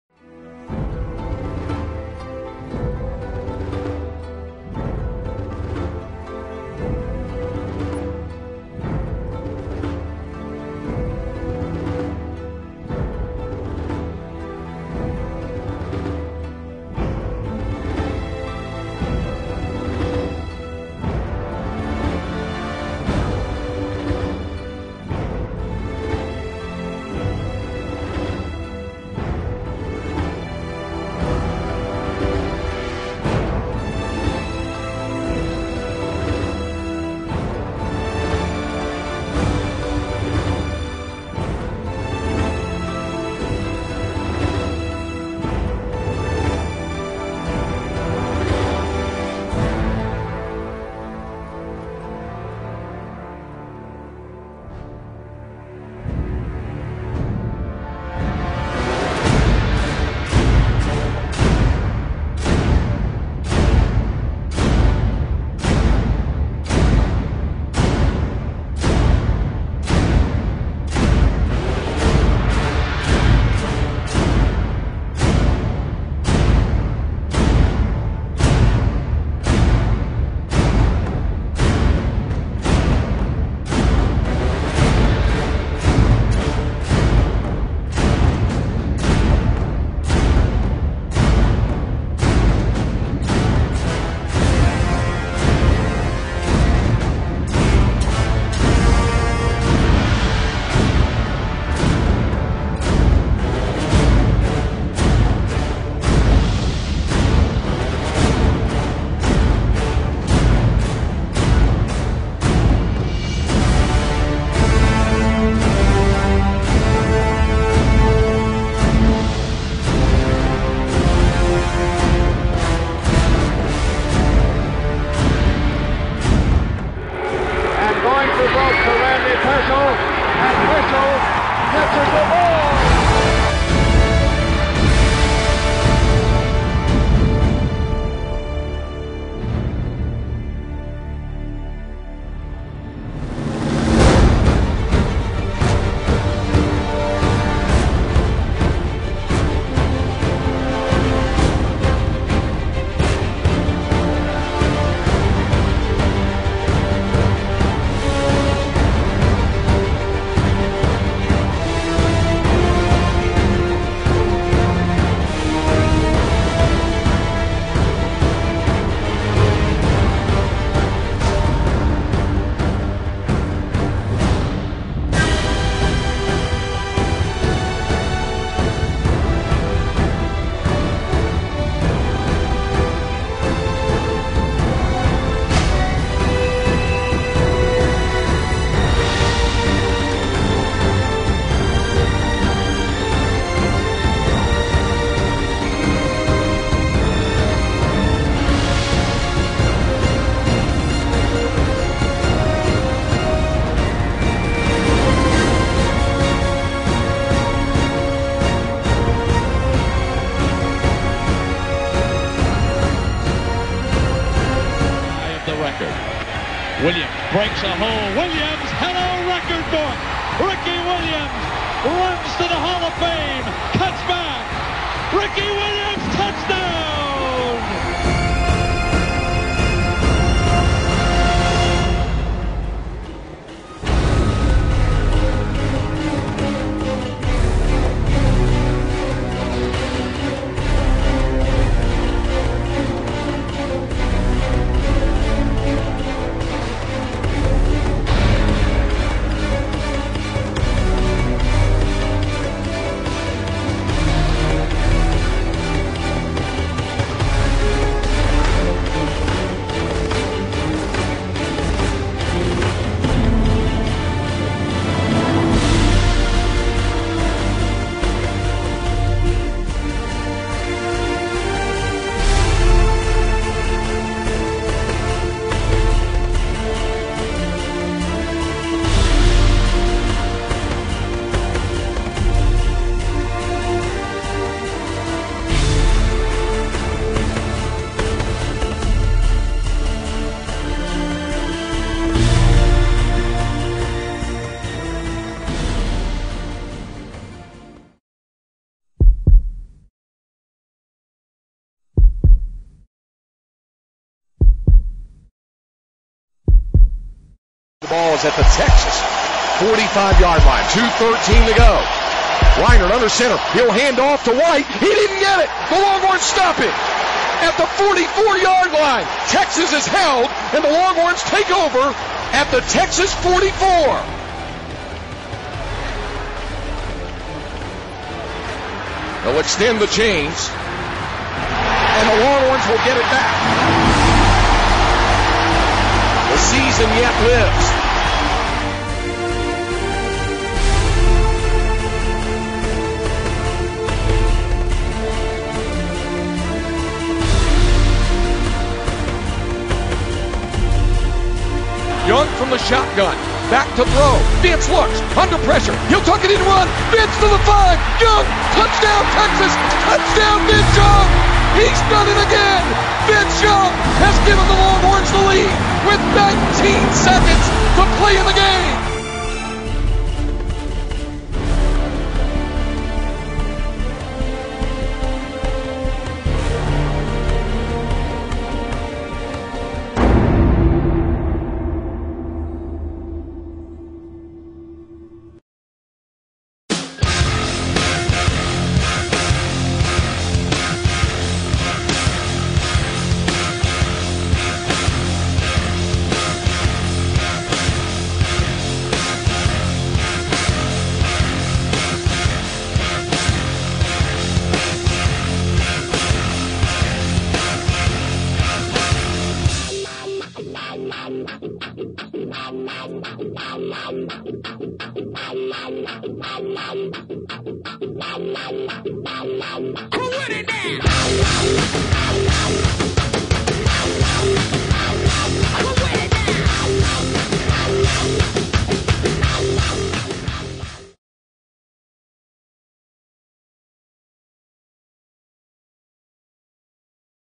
Texas Football History Intro Video
Video I recently made of the history of UT football. Could/should be played as an intro video before every Horns game, especially with the new "Godzillatron."